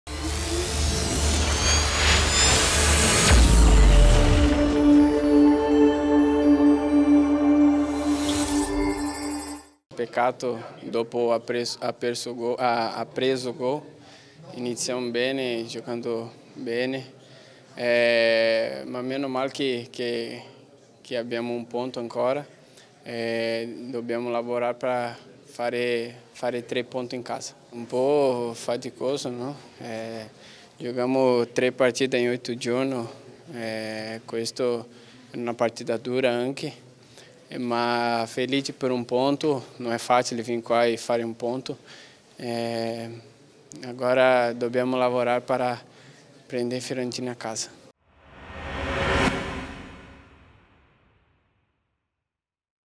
Chievo-Sampdoria, Fernando: «Conquistato un punto difficile»
Fernando analizza il punto conquistato sul campo del Chievo e volge già lo sguardo alla sfida di domenica al "Ferraris" con la Fiorentina capolista. «Dopo il vantaggio abbiamo giocato bene e potevamo chiudere la partita, peccato non esserci riusciti - spiega il metronomo brasiliano della Sampdoria -. Non è facile fare punti su questo campo e non dimentichiamo che era la terza partita in otto giorni. Ora pensiamo a fare bene in casa».